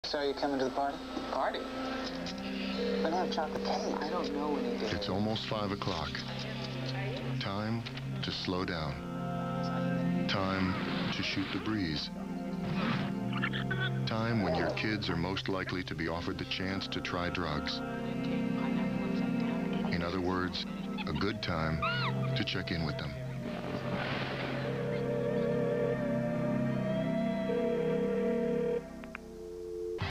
Anti-drug PSA, 1999
Spot telling how communication with your kids is "the anti-drug".